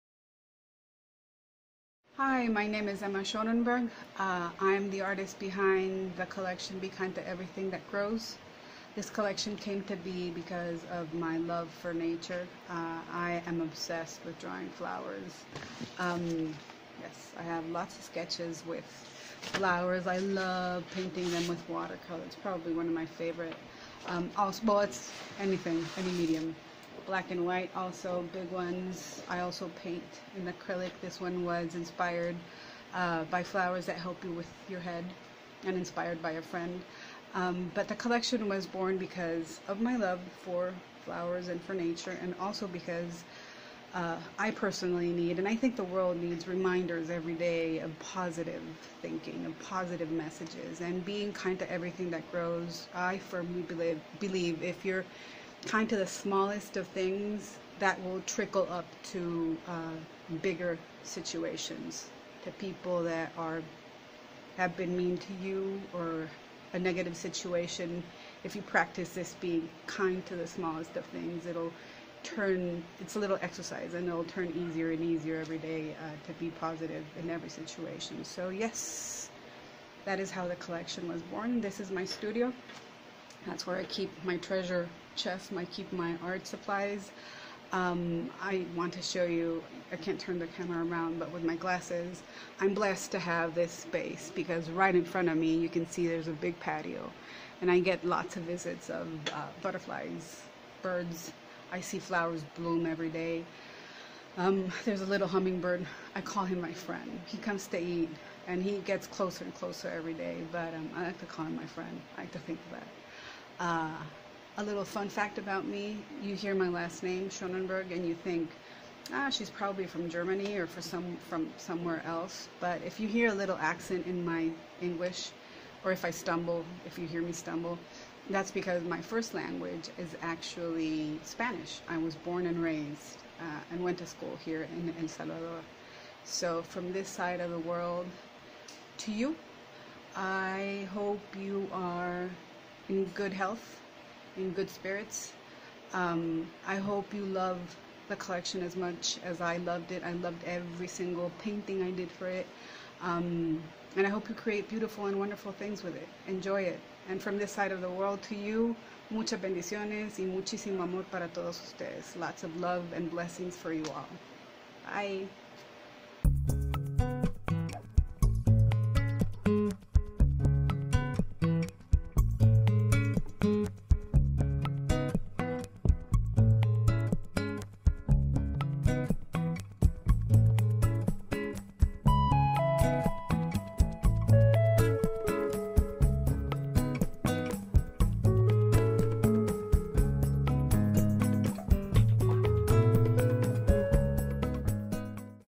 Michael Miller Fabrics' Emma Schonenberg Designer Showcase - Be Kind to Everything that Grows
SHIPPING FEBRUARY 2021
We are super excited to show off this great collection for
Michael Miller Fabrics.